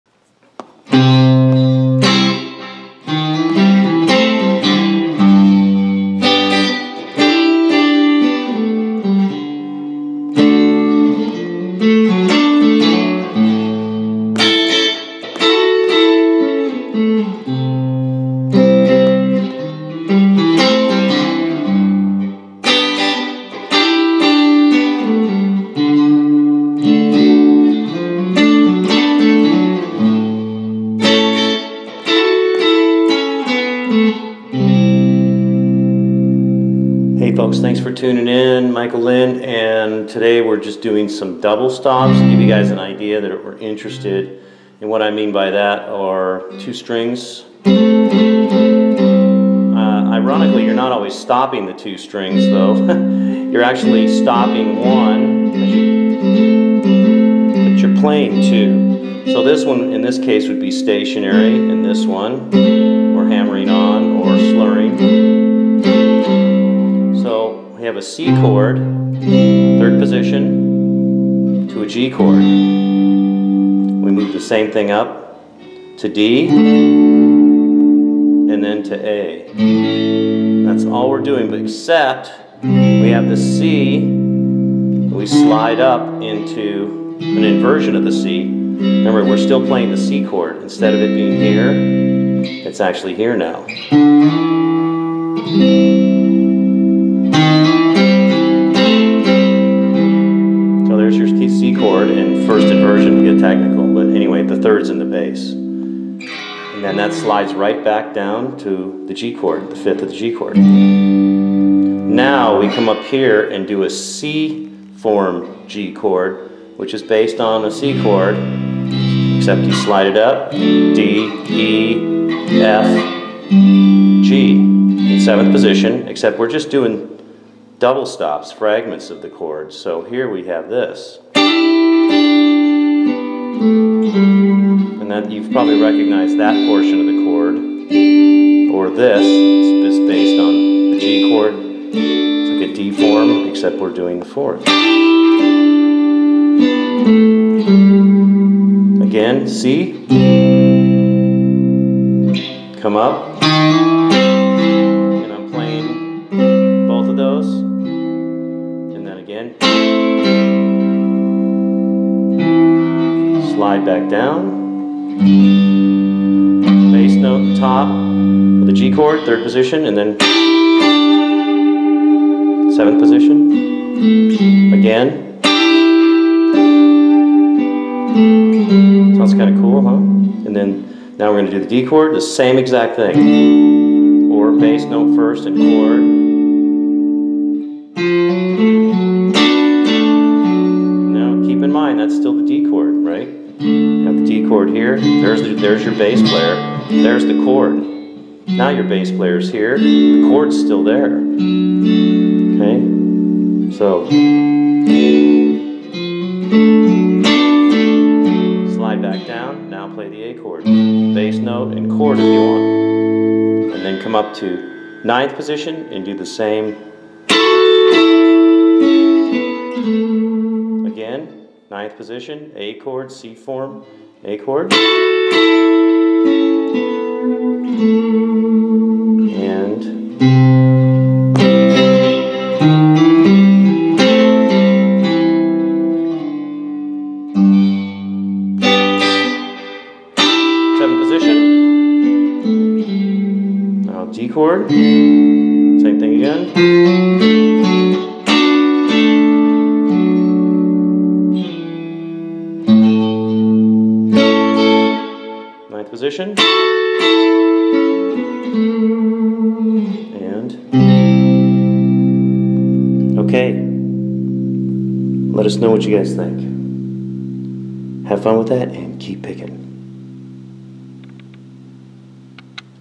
Learn the cool Hendrix groove tone of ~ {Double stops} ~ By Michael Lyn
Here is a 4 chord introduction to those wonderful 2 string jingle tones that all the greats use to win over our ears and enhance their playing.
These 2 string stops are used in too many styles to list including the great Hendrix, Trower, etc. and their use of them!